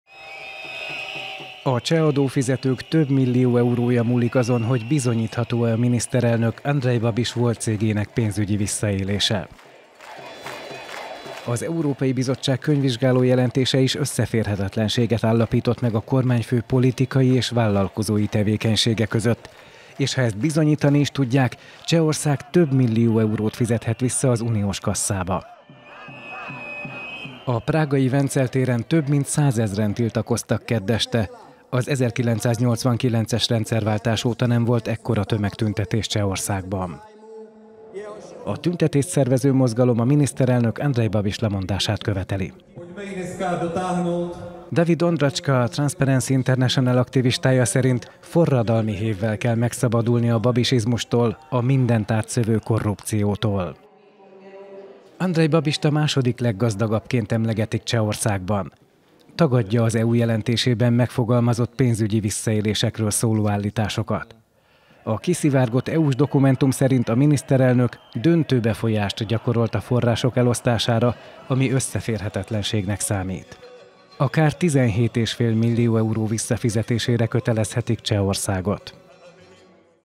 Menesztenék a kormányfőt a tüntető csehek
„Forradalmi hévvel kell megszabadulni a babisizmustól, a mindent átszövő korrupciótól!” - mondta a tüntetés szónoka Párágban.…